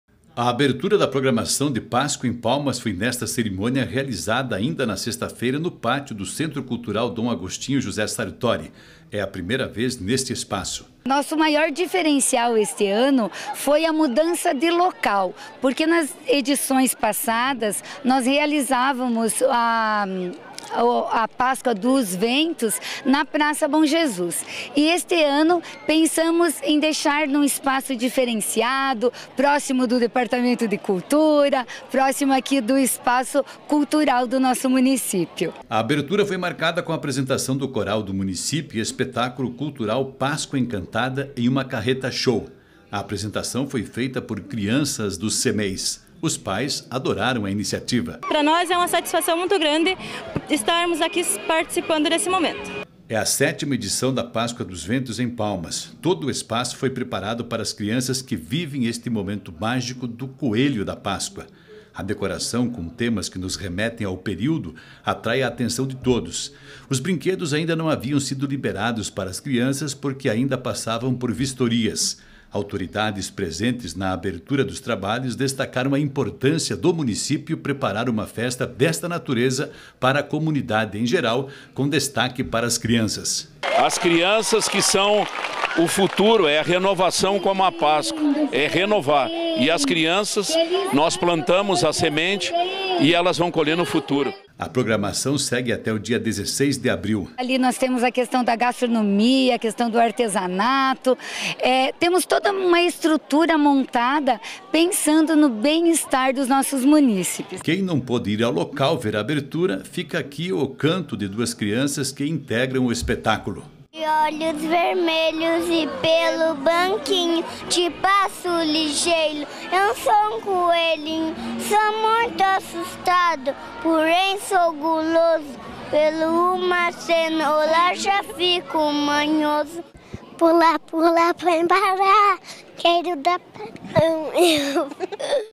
Palmas vive a programação da Páscoa
Eventos: Em Palmas, a programação da Páscoa já foi oficialmente aberta.